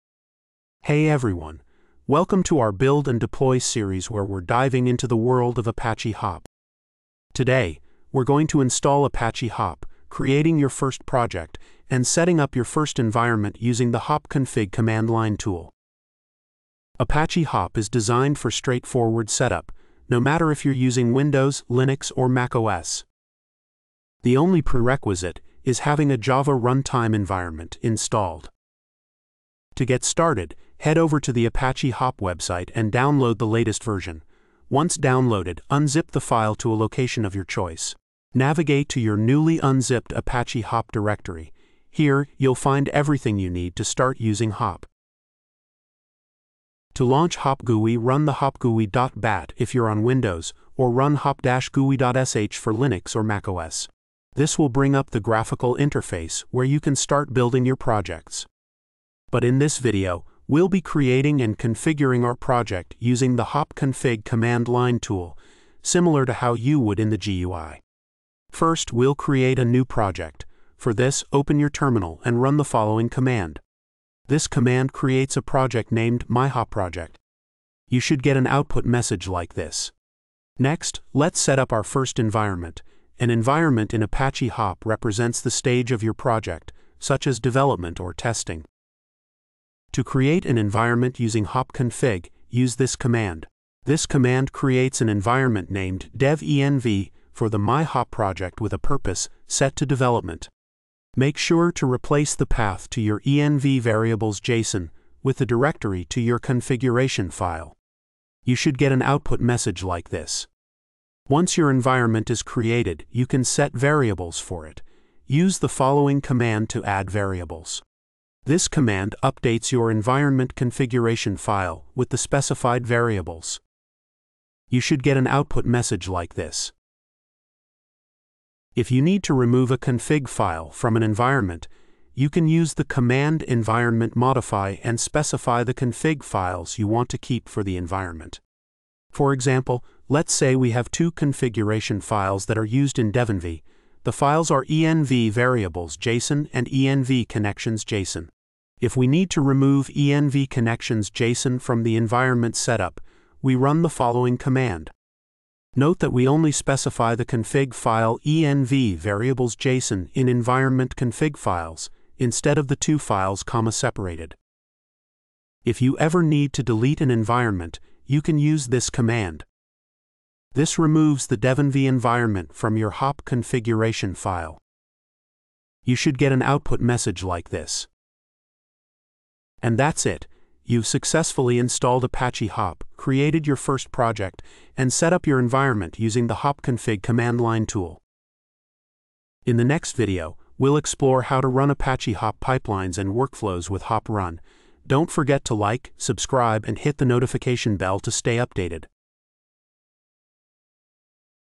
Build & deploy 5: First project and environment using Hop Conf
In this video, we continue our series on building and deploying data solutions with Apache Hop by creating our first project and environment using Hop Conf.

Learn how to:
- Set up a new project in Apache Hop, organizing your workflows and pipelines in a structured manner.
- Create and configure an environment to manage different execution contexts, such as development, testing, and production.
- Use Hop Conf to manage your configuration files and settings, ensuring smooth project and environment transitions.

📚 INDEX:
0:00 Introduction
0:16 Supported OS
0:24 Prerequisite
0:29 Install
0:47 Windows
0:52 macOS or Linux
0:59 Project setup
1:29 Environment setup
2:59 Summary